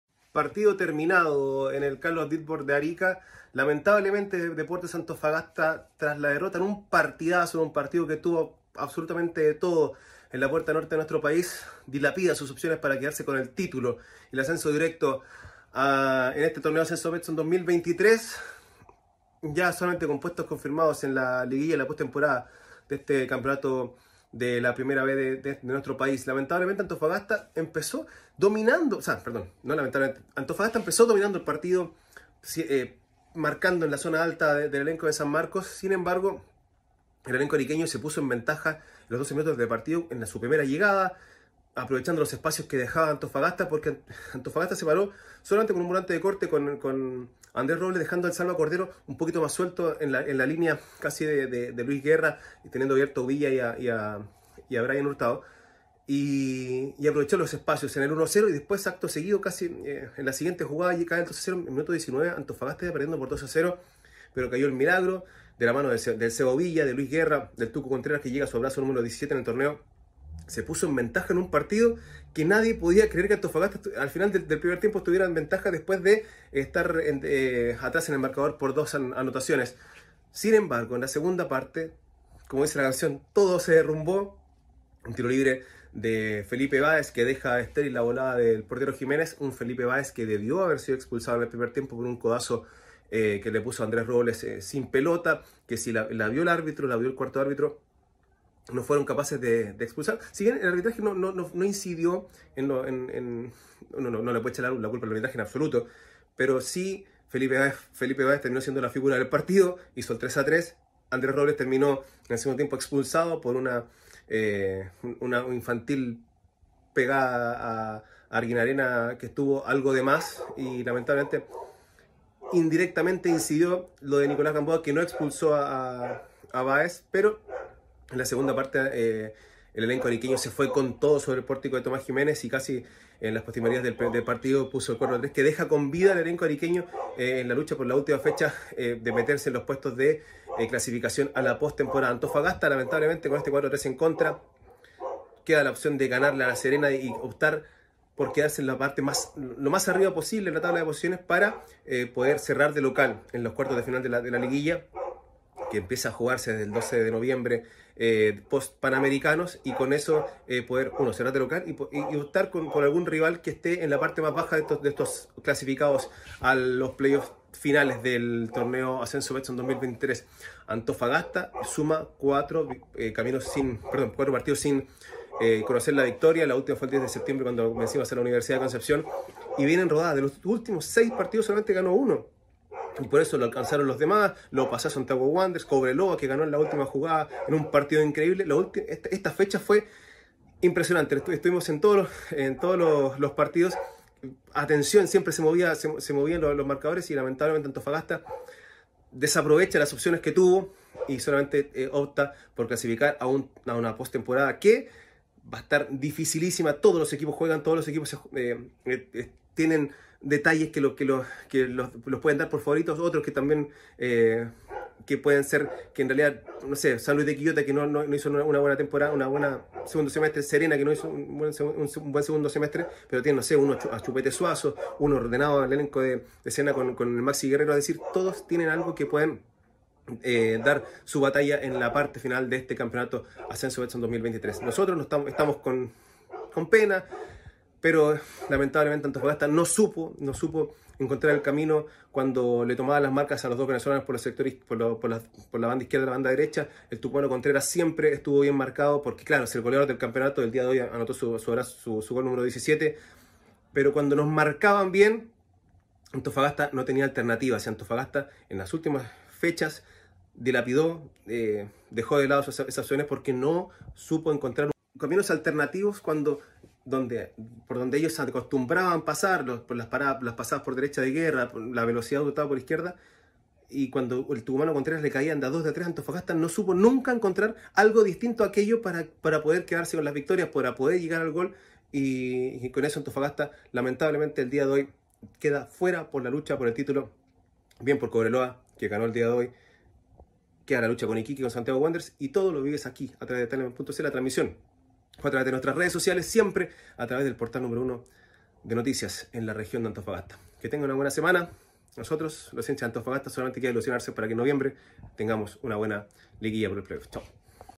"De los últimos 6 partidos el CDA solo ganó 1": Análisis tras derrota ante Arica
ColumnaTL | "De los últimos 6 partidos el CDA solo ganó 1. Por eso nos alcanzaron los demás equipos": Mario Pérez Bugueño, comentarista deportivo, tras derrota de Deportes Antofagasta ante San Marcos de Arica, lo que lo deja fuera del ascenso directo